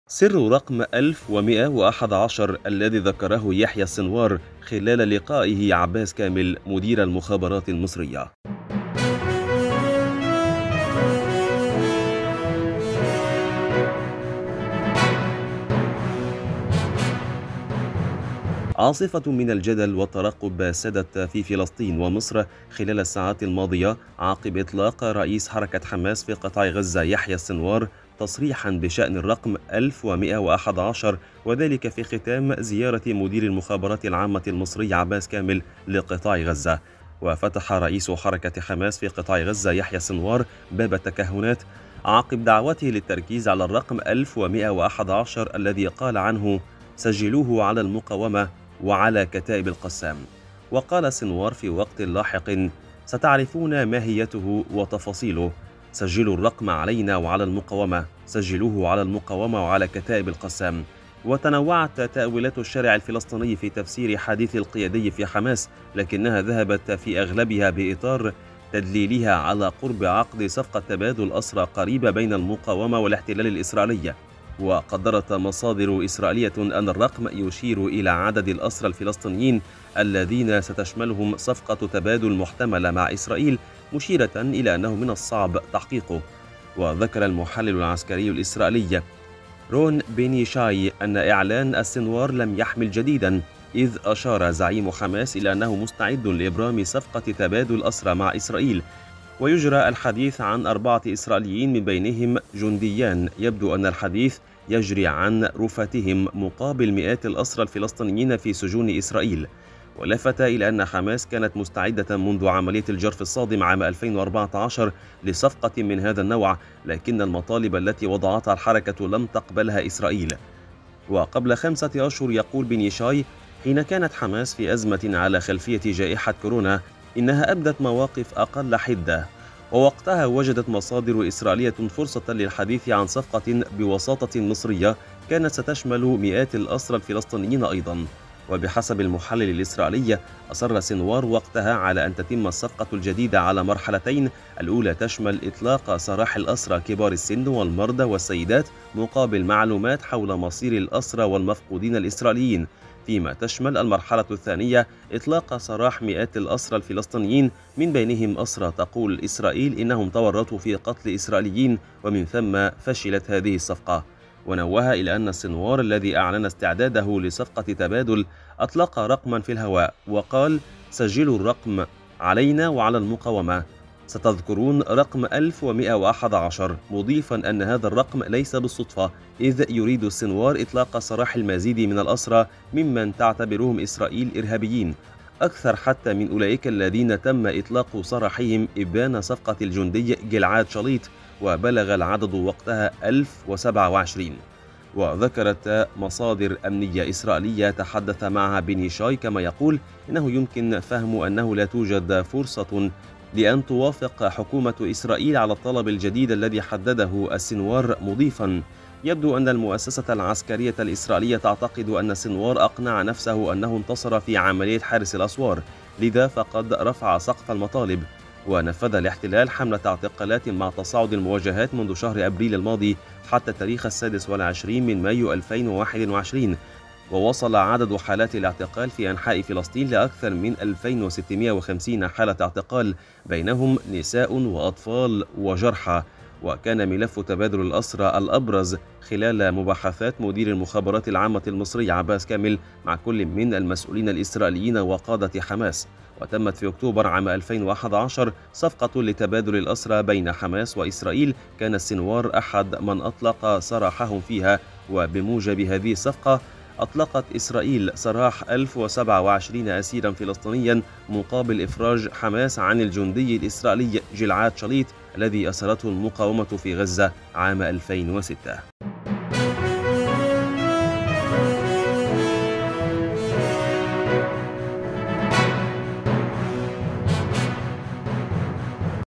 سر رقم "1111" الذي ذكره يحيى السنوار خلال لقائه عباس كامل مدير المخابرات المصرية
عاصفة من الجدل والترقب سادت في فلسطين ومصر، خلال الساعات الماضية عقب أطلاق رئيس حركة حماس في قطاع غزة يحيى السنوار تصريح بشأن الرقم 1111 وذلك في ختام زيارة مدير المخابرات العامة المصري عباس كامل لقطاع غزة.